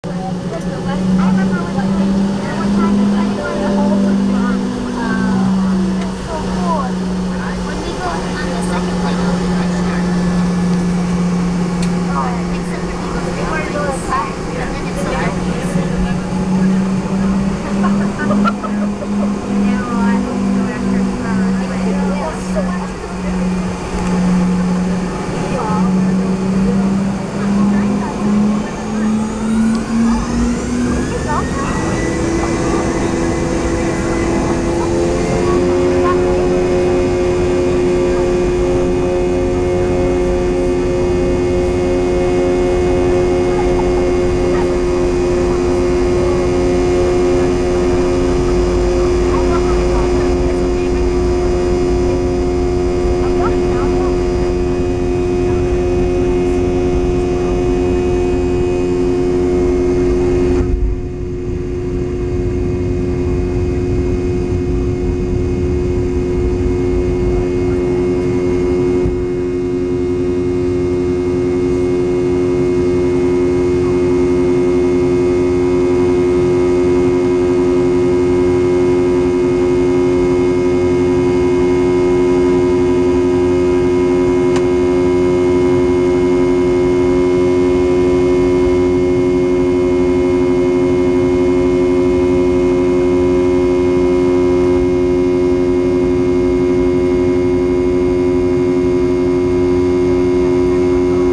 Airtran 717-200 Take-off from Greater Rochester Int'l (KROC)
Our Airtran 717 flight to Baltimore departing Rochester 3 hours late.